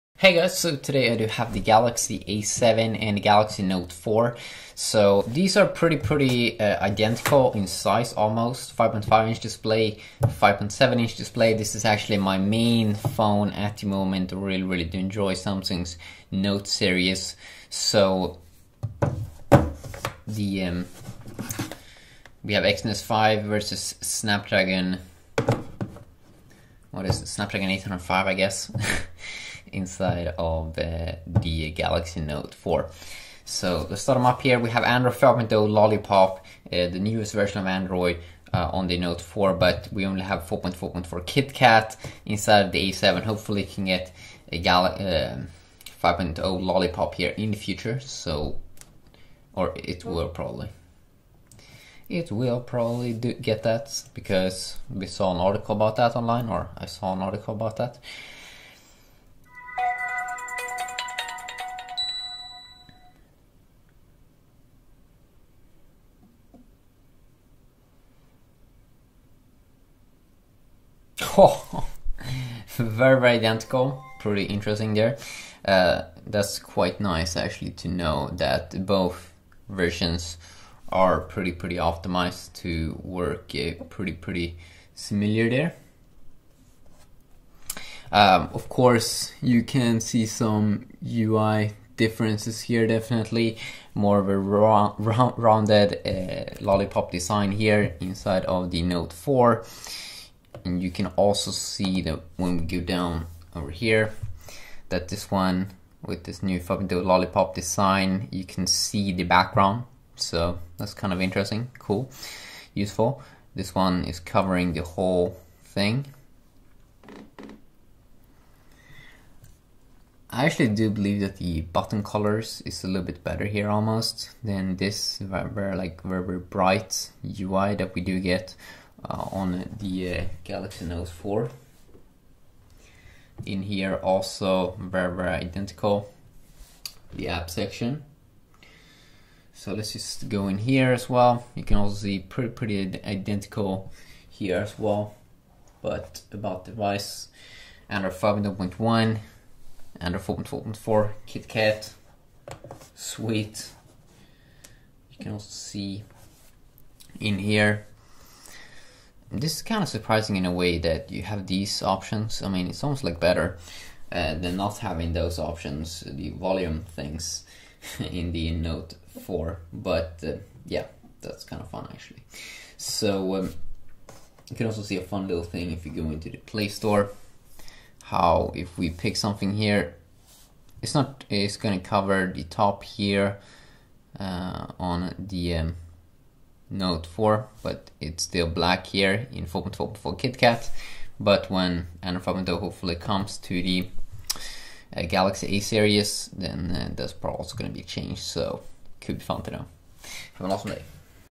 Samsung Galaxy A7 vs Samsung Galaxy Note 4 -Which Is Faster ? 4K
Which is faster samsung galaxy A7 or galaxy note 4 see this video to nowing and share it if you like :)
see you .